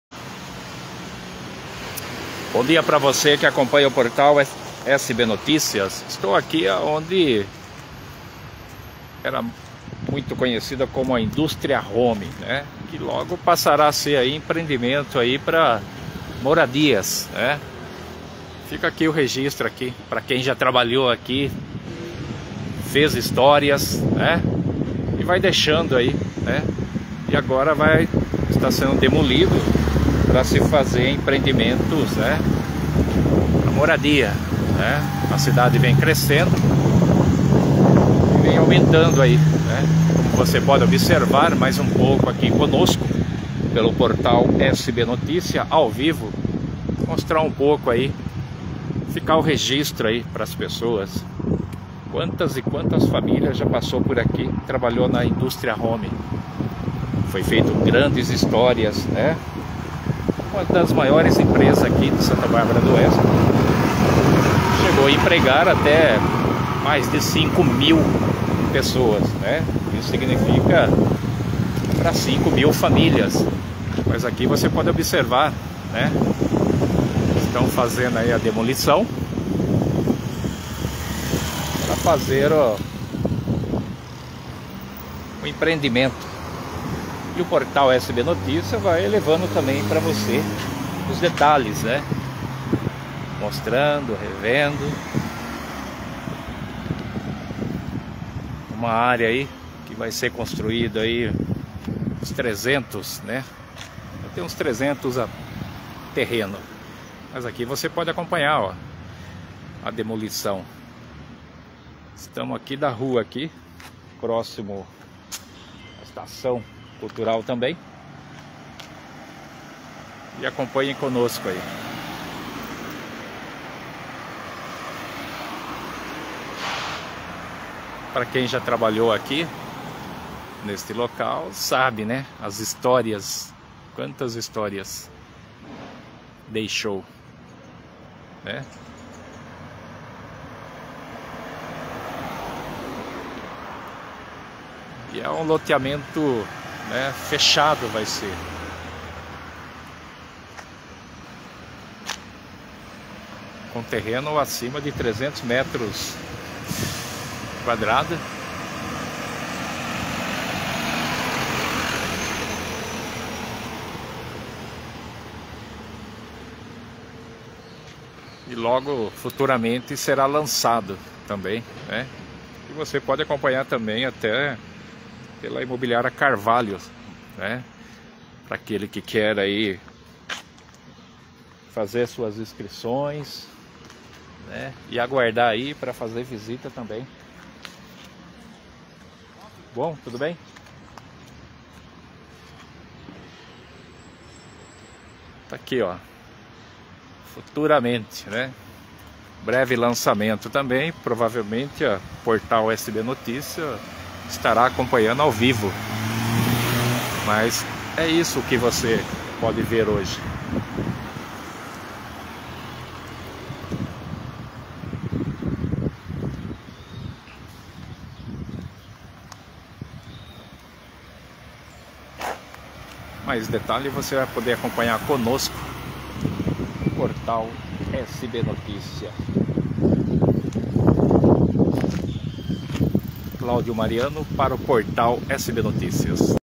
Empresa da Industria Romi, localizada na Av. Pérola Byington, Vila Romi
Empresa da Industria Romi, localizada na Av. Pérola Byington, Vila Romi, em Santa Bárbara d'Oeste está sendo demolida para a construção de um novo empreendimento na cidade.